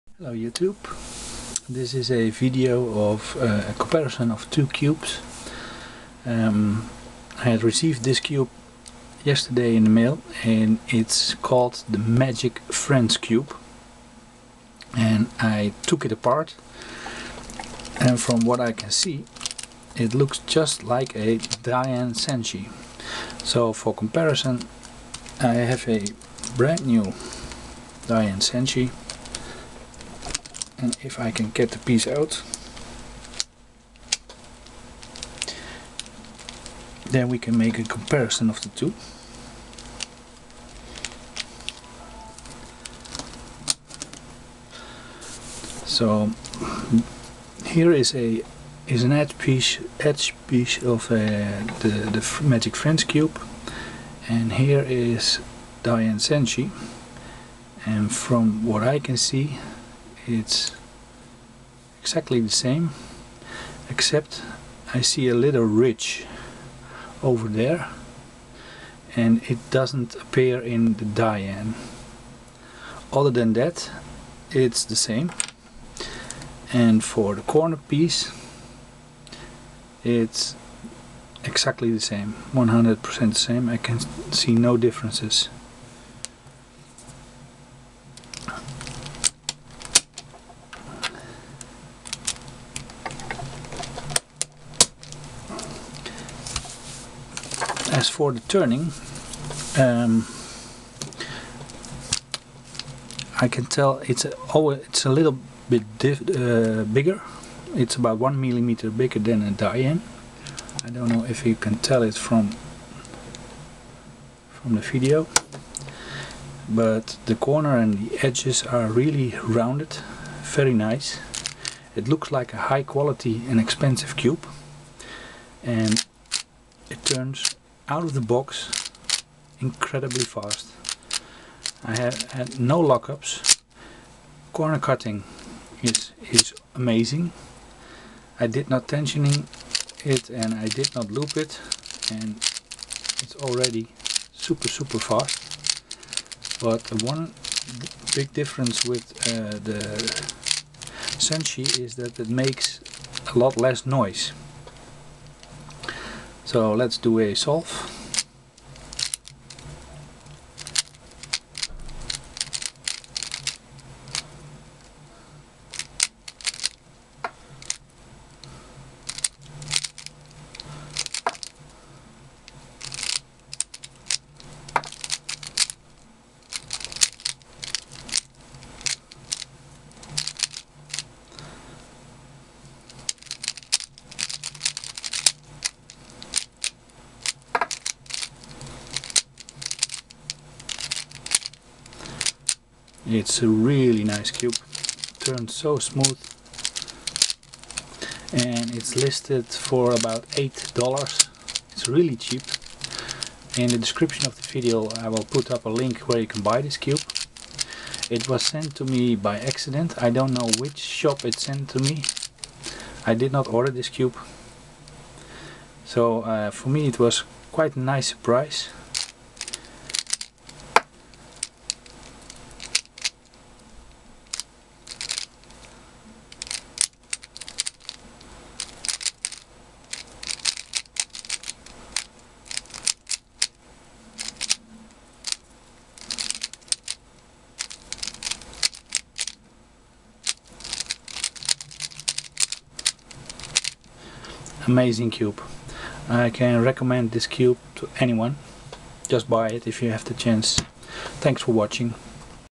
Magic friends cube review
Review compare between Dayan Zhanchi and Magic friend.